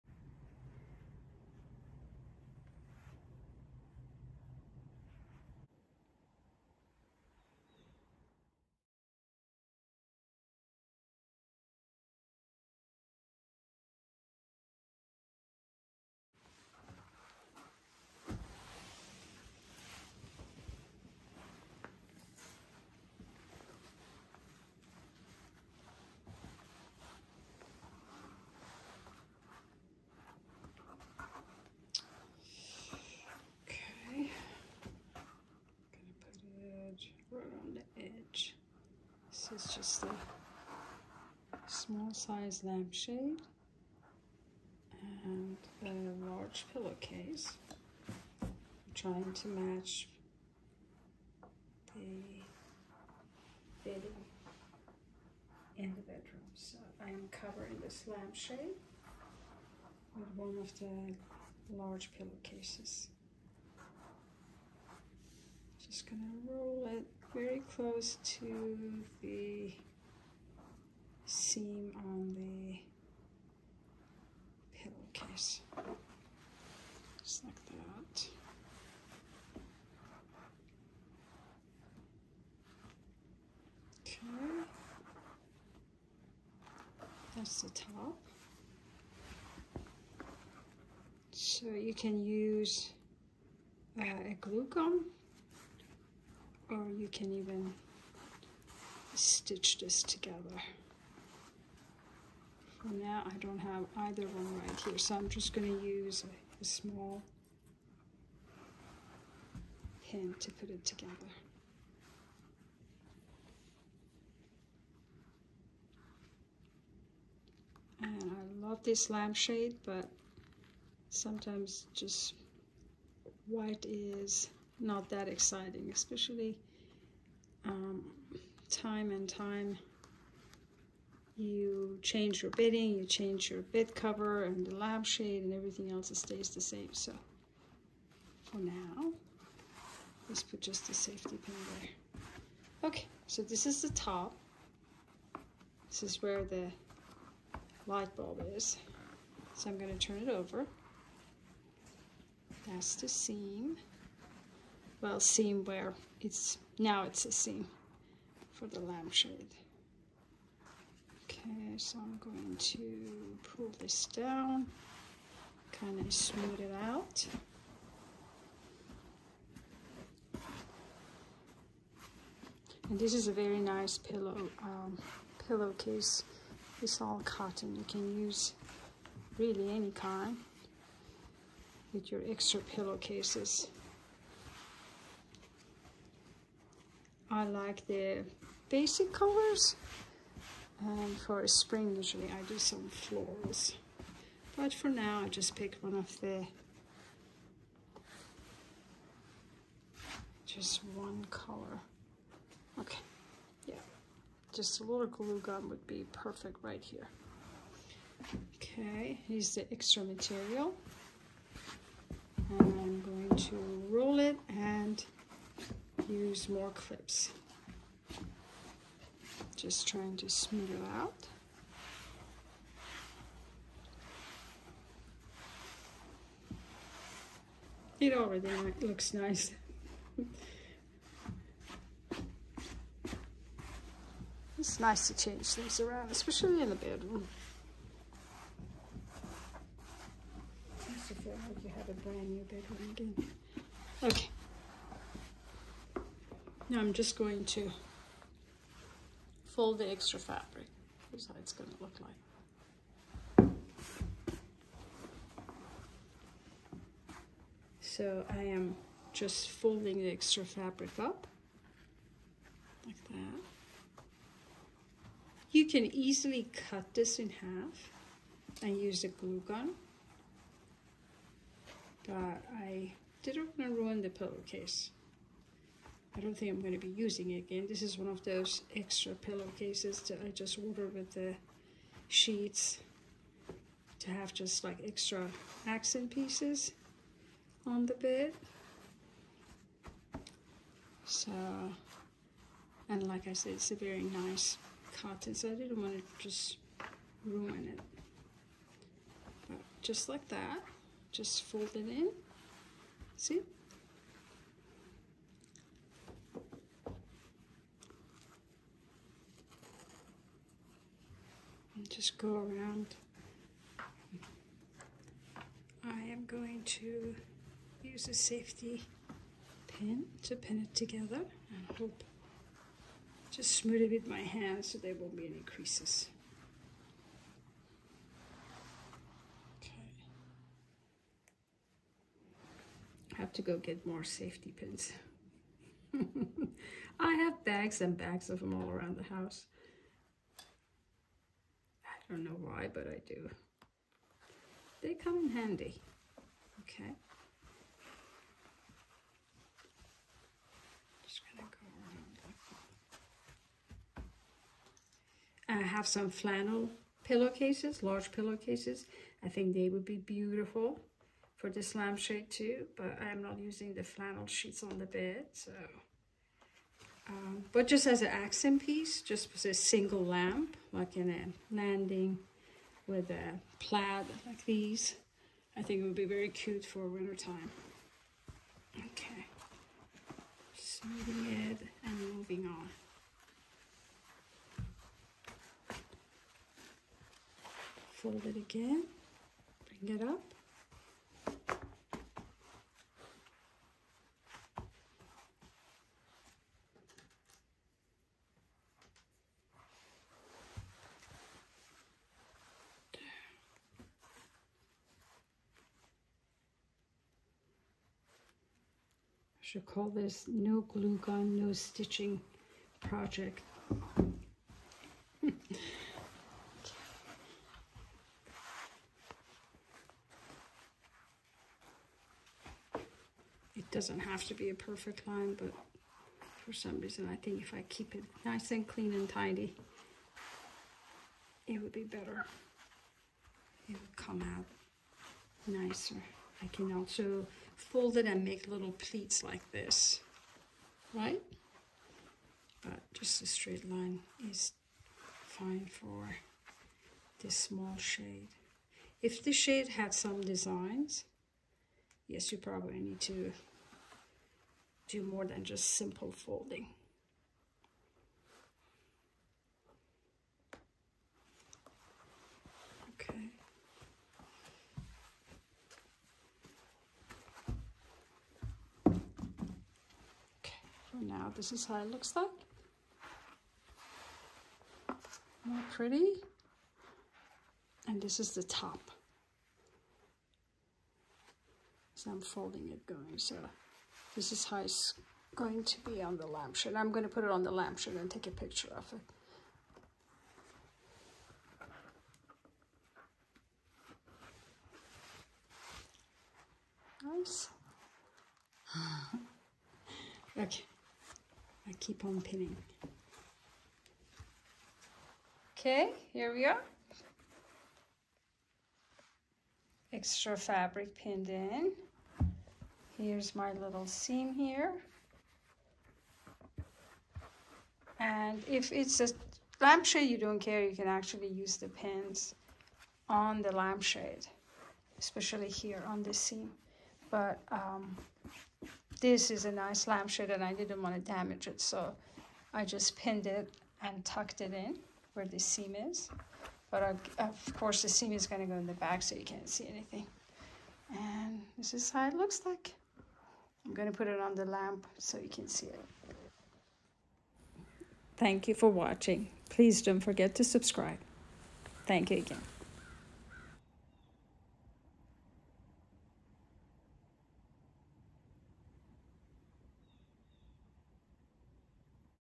Lamp shade Hacks step by step
Using a large pillowcase, I am covering a small shade for a new look. This is also a great idea if you would like to match the lampshades in your bedroom with your bed linens by ordering large size pillowcases. To watch the entire process please watch the following video.